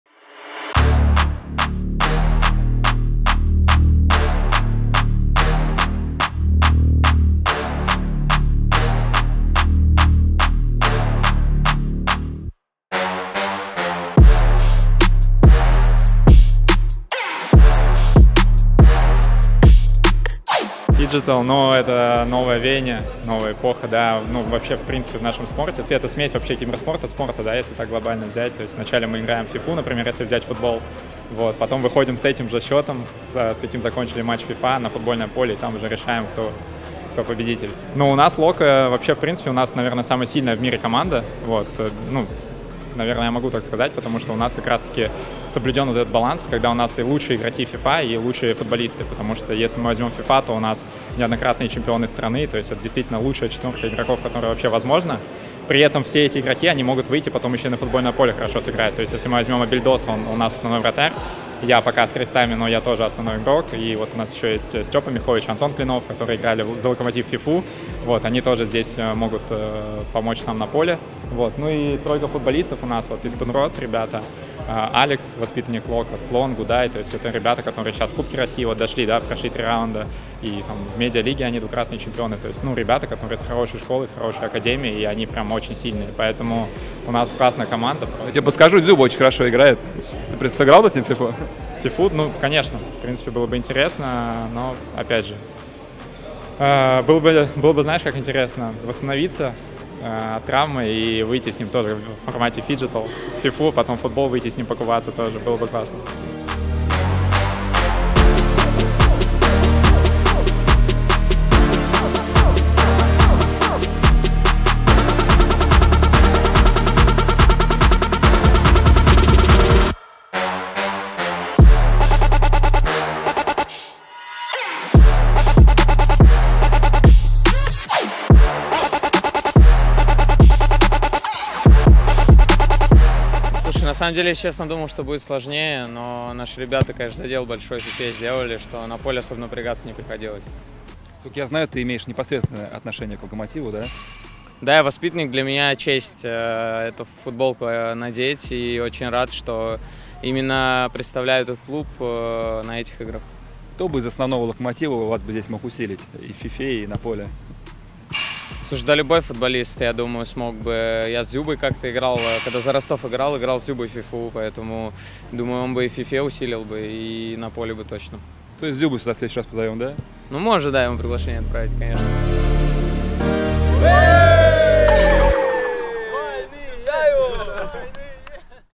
«Локомотив» выиграл отборочный этап фиджитал-турнира «Игры будущего»
«Локомотив» — победитель первого регионального отборочного этапа фиджитал-турнира «Игры будущего» 🏆

Наши парни во главе с Робертом «Ufenok77» Фахретдиновым с большим преимуществом одолели соперников и завоевали победу. «Локо» царил и на футбольном поле, и в игре FIFA 👏

Поздравляем парней, следим за их выступлениями, болеем и ждём в финале! 🙌

Состав «Локо»: Алекс Стиль, Слон, Гудай, Фанкбраза, Кленов, Абельдос, Роберт Уфенок.